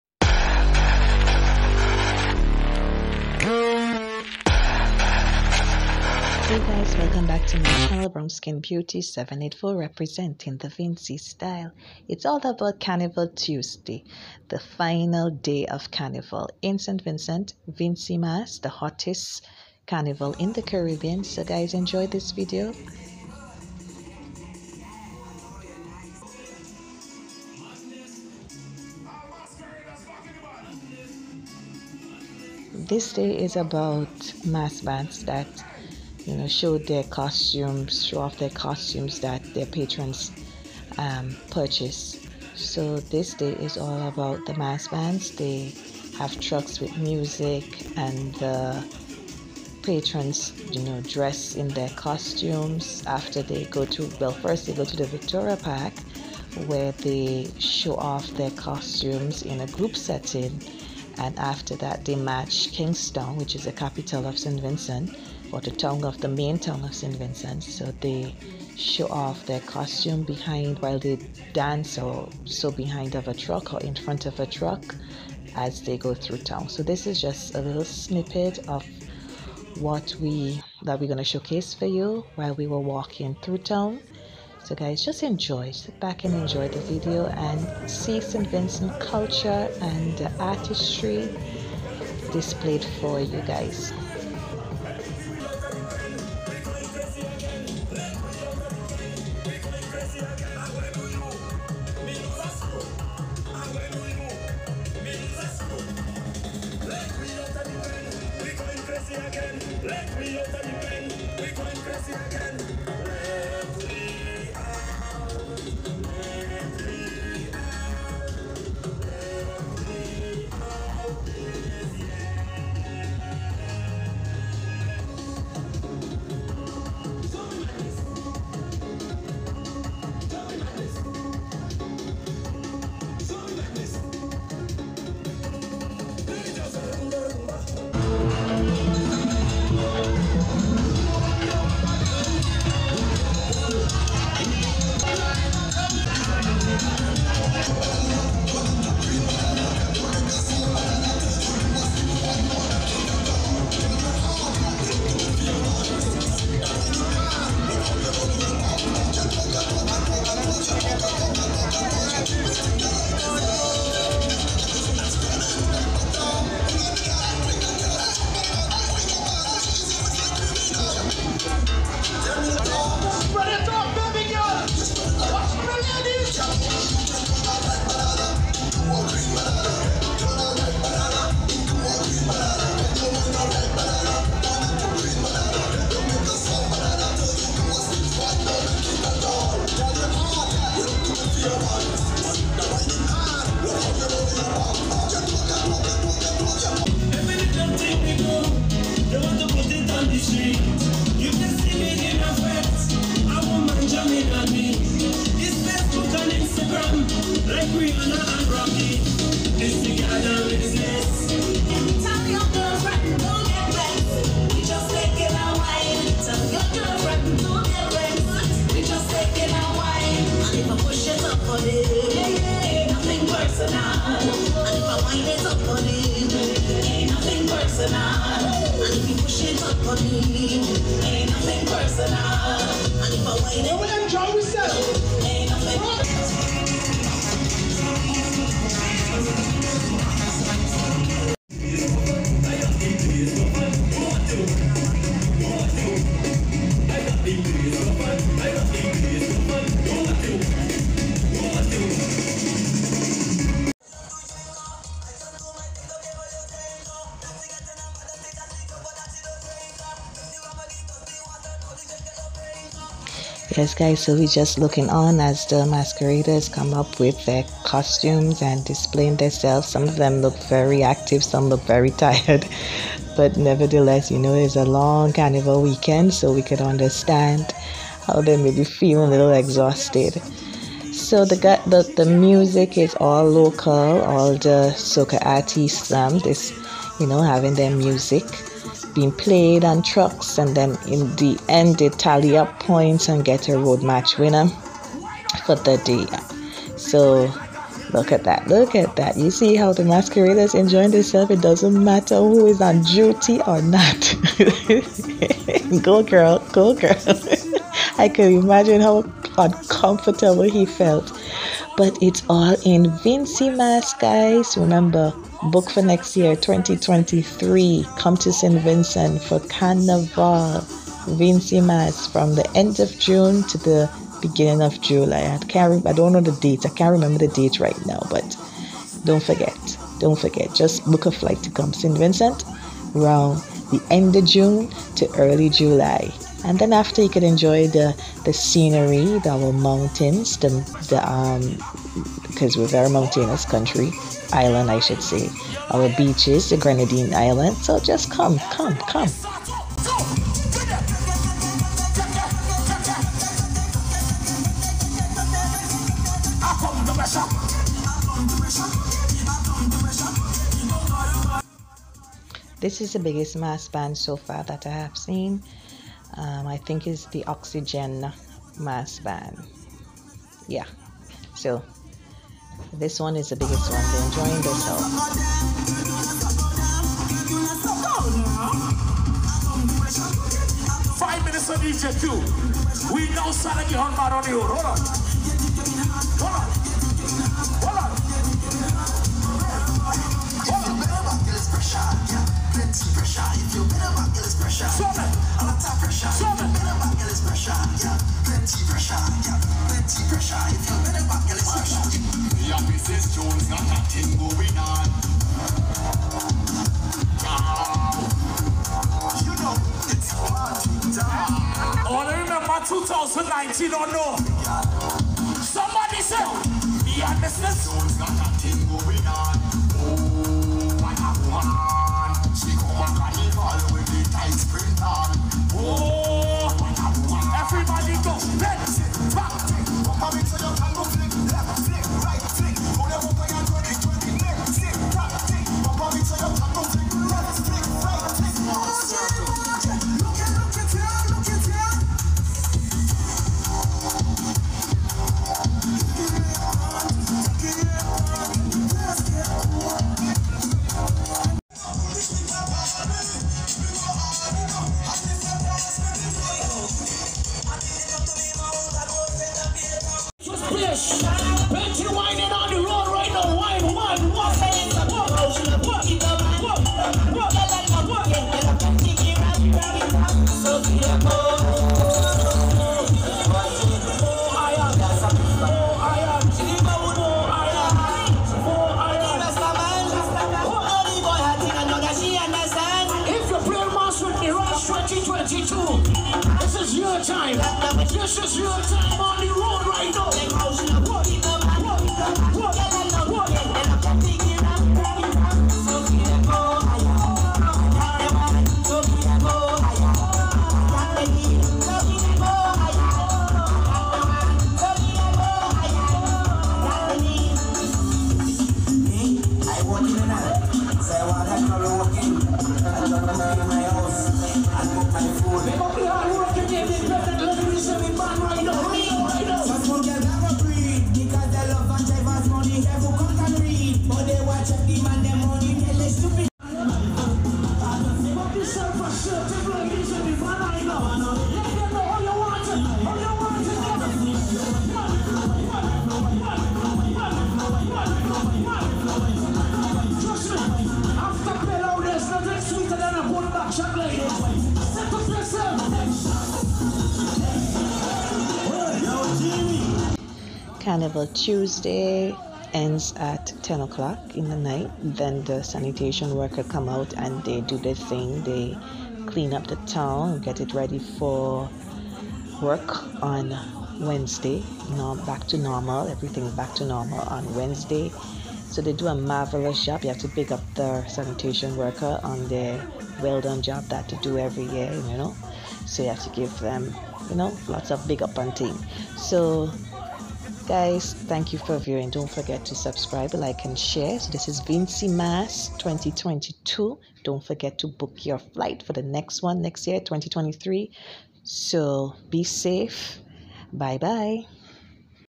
Carnival Tuesday (VINCY MAS 2022)
It's carnival season in St Vincent and the Grenadines, this is just a short video about the final day of vincy mas the hottest carnival in the Caribbean, so make reservations for 2023.😀😀👍🏾